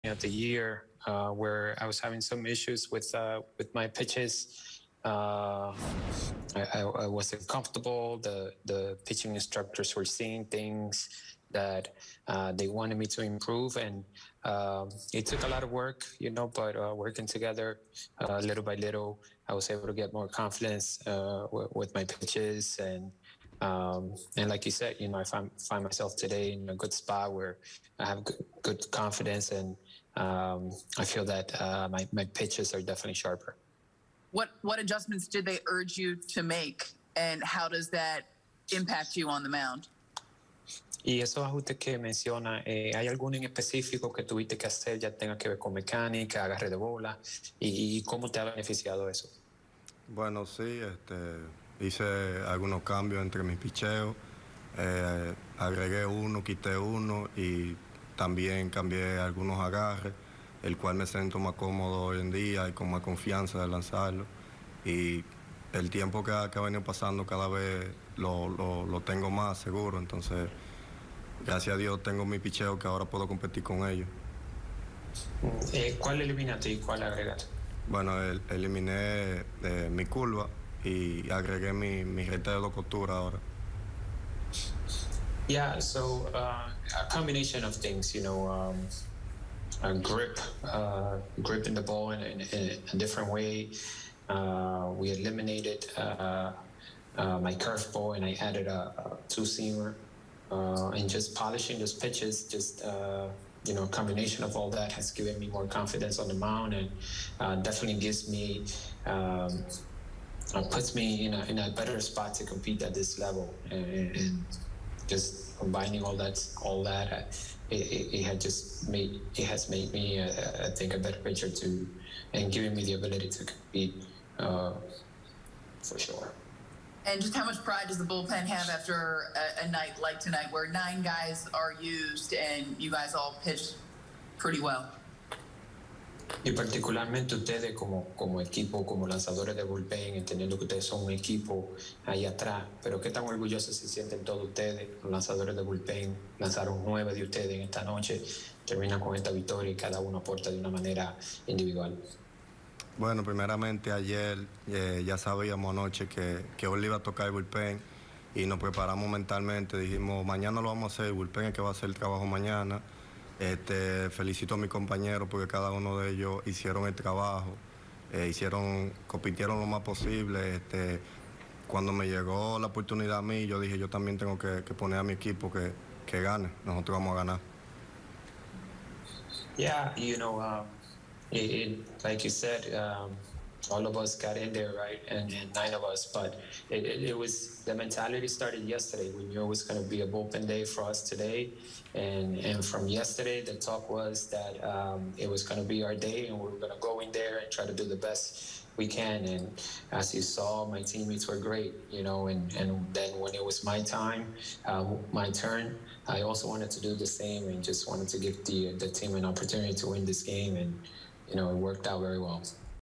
Albert Abreu reflects on his outing against Seattle
Albert Abreu shares his thoughts on his performance on the mound as he records his first win in the MLB.